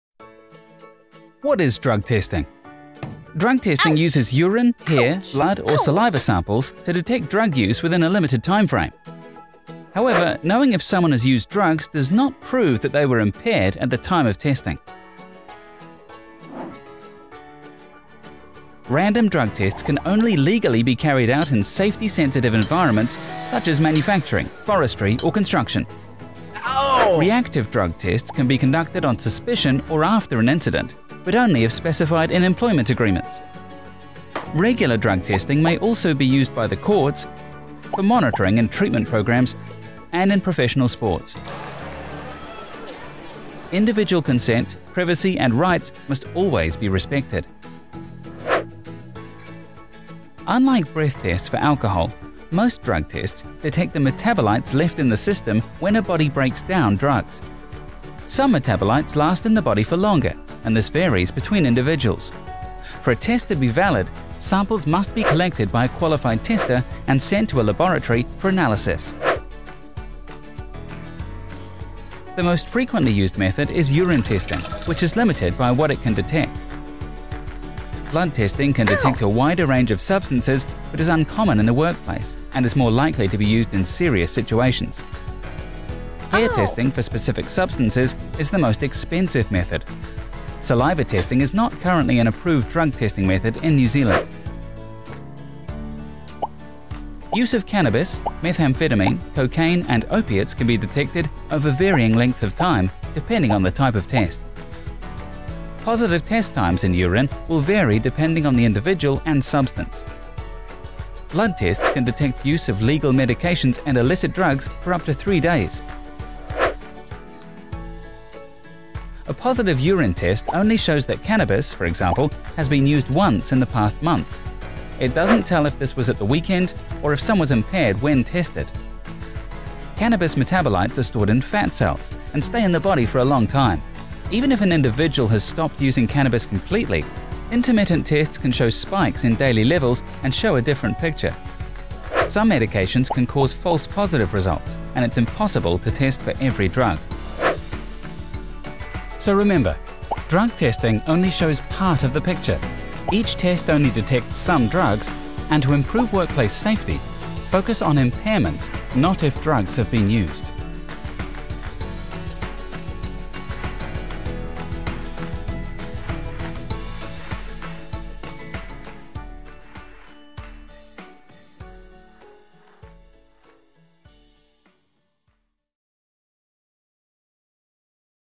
What is Drug Testing? an explainer
Get clear about what drug testing is, and what it's limits are.

This animated video makes it clear what
- how it works
- types of drug tests
- what can be tested
- when it can be used
- what the limits are.

This animation was produced in early 2018 for anyone in New Zealand looking for accurate information about drug testing.